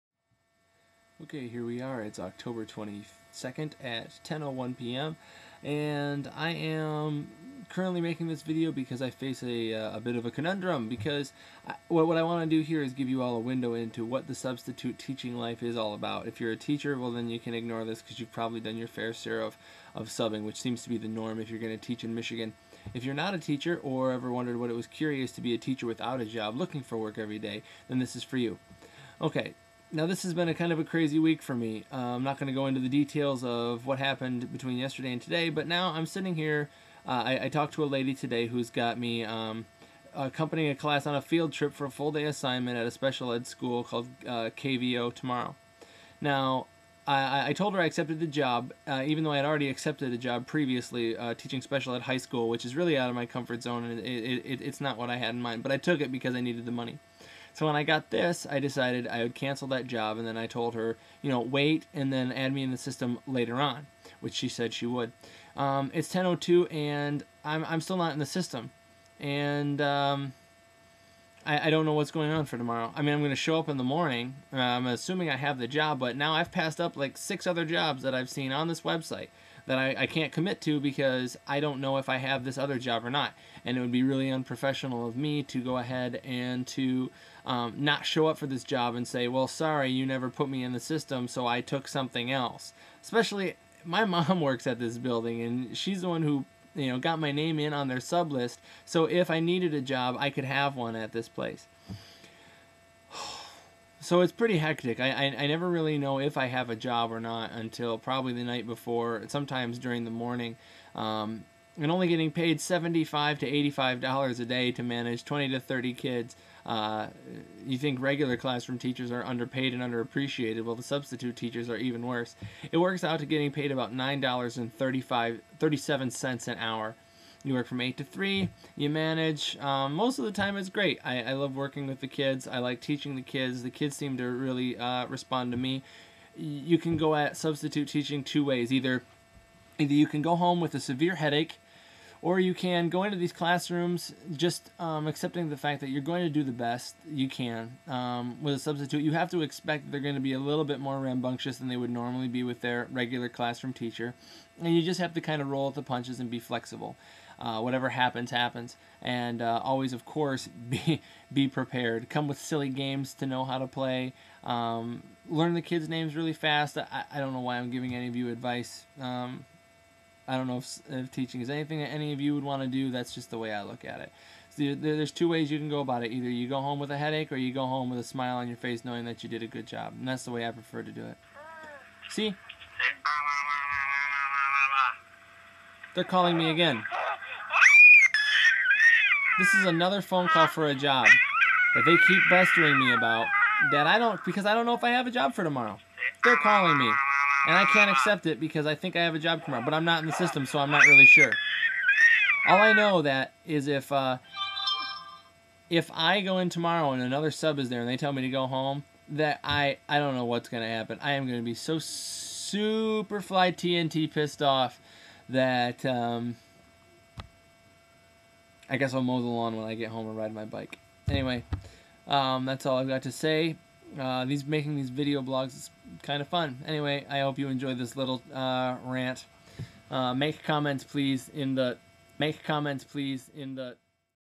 The perils of Substitute teaching.mov
Wherein I describe the difficulty of finding reliable work as a substitute teacher.